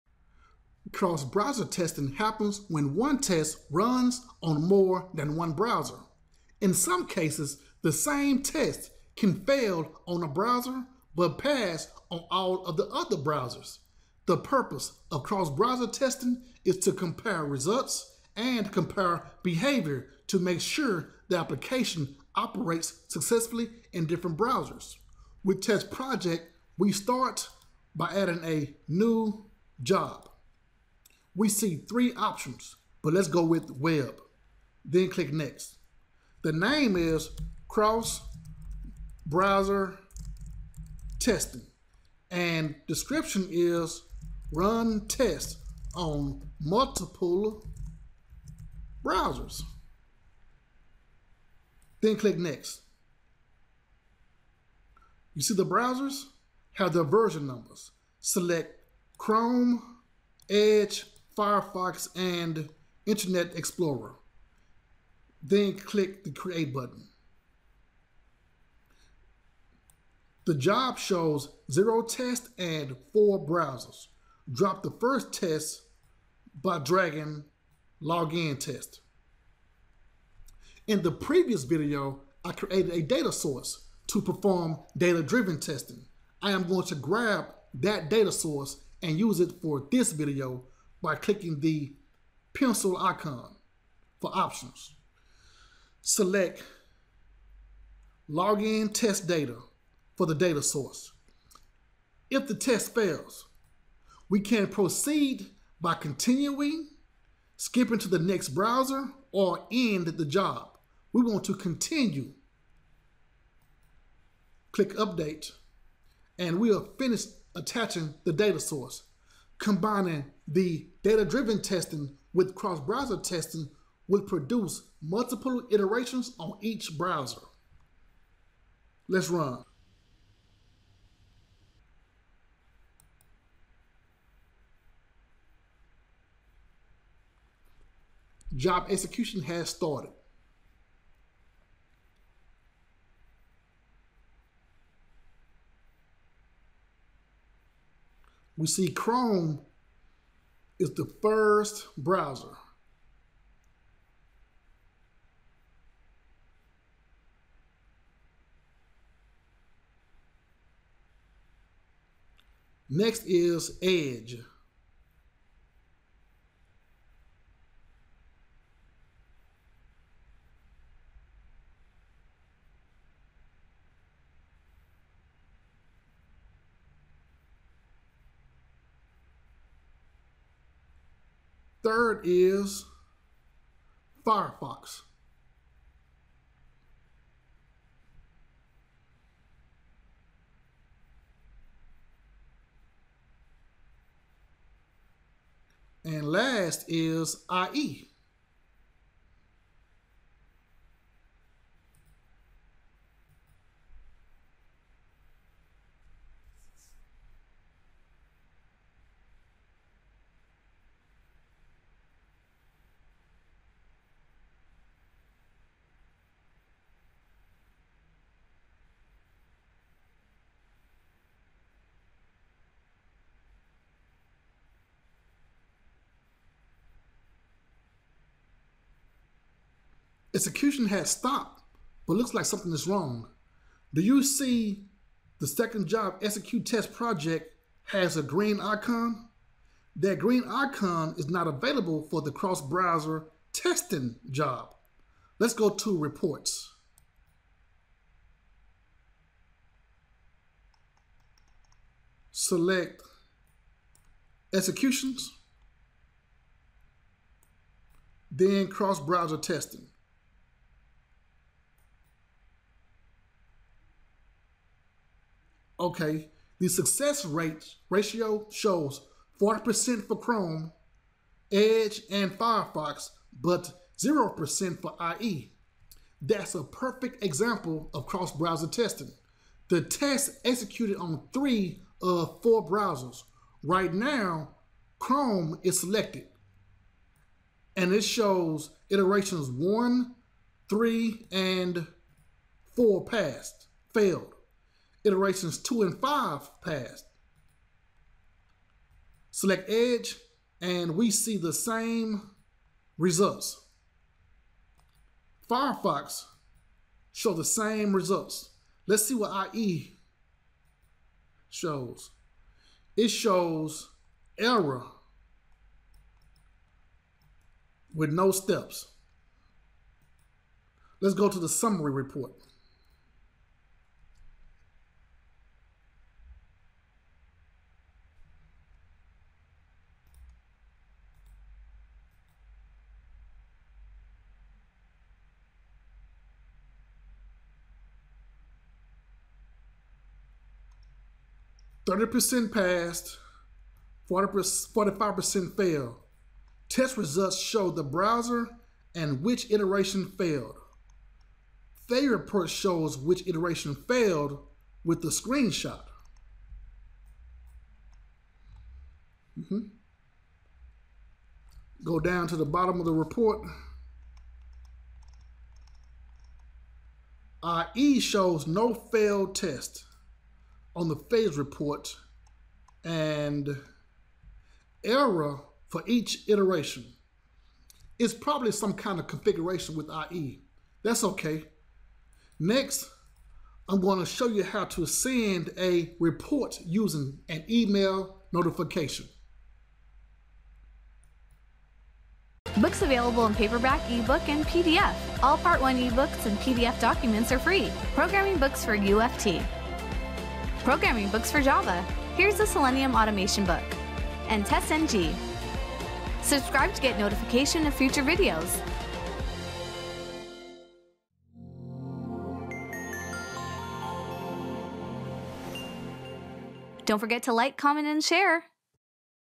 ✔ Cross Browser Testing With TestProject | (Video 128)
Cross Browser Testing With TestProject

► Transcript
Cross Browser Testing happens when 1 test runs on more than 1 browser. In some cases, the same test can fail on a browser but pass on all of the other browsers. The purpose of Cross Browser Testing is to compare results and compare behavior to make sure the application operates successfully in different browsers.

With TestProject, we start by adding a new job. We see 3 options but let’s go with Web then click Next. The Name is Cross Browser Testing and Description is Run Test On Multiple Browsers then click Next. You see the browsers have their version numbers. Select Chrome, Edge, Firefox, and Internet Explorer then click the Create button. The job shows 0 Test and 4 Browsers. Drop the first test by dragging Login Test.

In the previous video, I created a Data Source to perform Data Driven Testing. I am going to grab that Data Source and use it for this video by clicking this pencil icon for Options. Select Login Test Data for the Data Source. If the test fails, we can proceed by continuing, skipping to the next browser, or End the job. We want to Continue. Click Update and we are finished attaching the Data Source. Combining the Data Driven Testing with Cross Browser Testing will produce multiple iterations on each browser. Let’s Run. Job Execution has started. We see Chrome is the first browser, Next is Edge, 3rd is Firefox, and last is IE. Execution has stopped but looks like something went wrong.

Do you see how the 2nd job Execute TestProject has a green icon? That green icon is not available for the Cross-Browser Testing job. Let’s go to Report. Select Executions then Cross Browser Testing.

Okay, the Success Ratio shows 40% for Chrome, Edge, and Firefox but 0% for IE.

That’s a perfect example of Cross Browser Testing. The test executed on 3 of 4 browsers. Right now, Chrome is selected and it shows Iterations 1, 3, and 4 Failed. Iterations 2 and 5 Passed. Select Edge and we see the same results. Firefox shows the same results. Let’s see what IE shows. It shows Error with no steps.

Let’s go to the Summary Report. 30% Passed / 45% Failed.

Test Results show the browser and which Iteration Failed. Failure Report shows which Iteration Failed with a screenshot. Go down to the bottom of the report. IE shows No failed test on the Failures Report and Error for each iteration. It’s probably some kind of Configuration with IE. That’s okay. Next, I’m going to show you how to send out a report using an Email Notification.